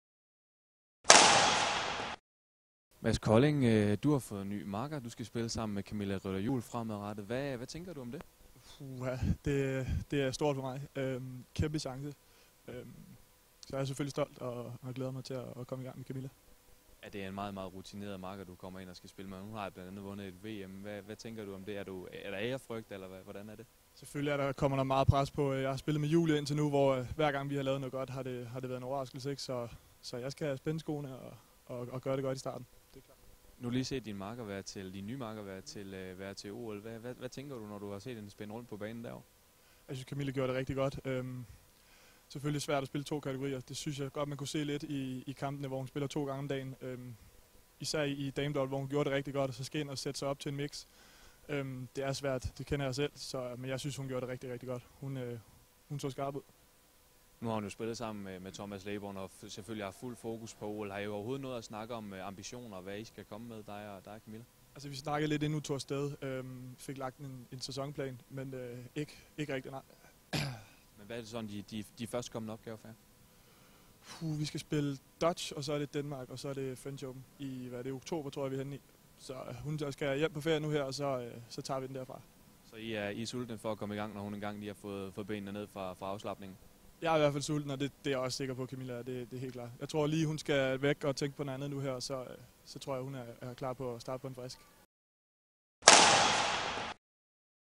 Kolding: Det er kæmpe stort
Mads Kolding skal fremover spille sammen med Kamilla Rytter Juhl, og det er noget, det unge mixedtalent glæder sig til.